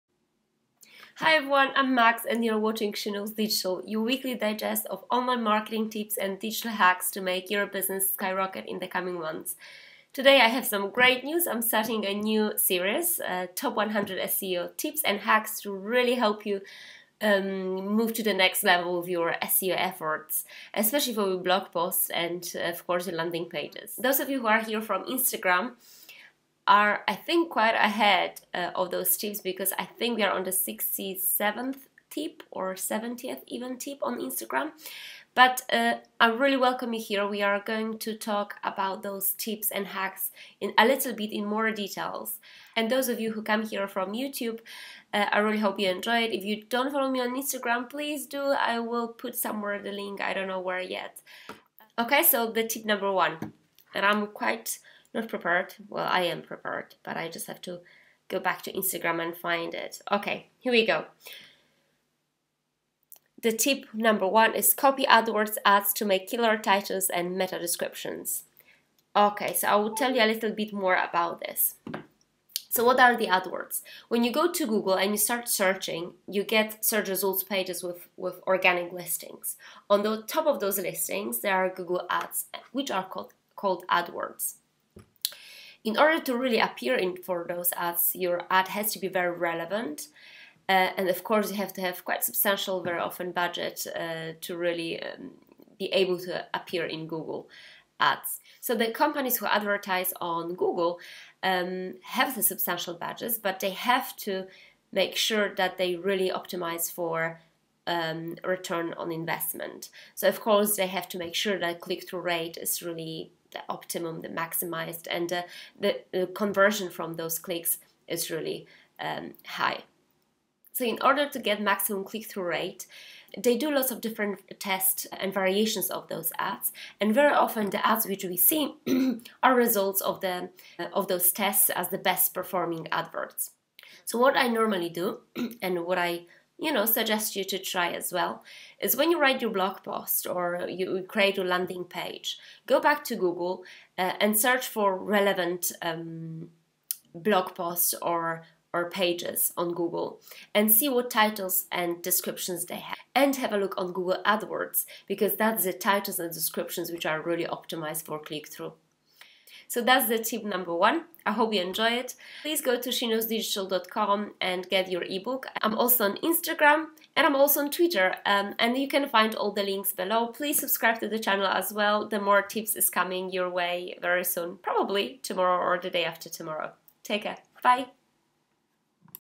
SEO Tip 1. Copy AdWords to make awesome meta titles and descriptions.
This first episode of the Top 100 SEO Tips series! The SEO Tip/ Hack no 1. Copy AdWords to make awesome meta titles and descriptions.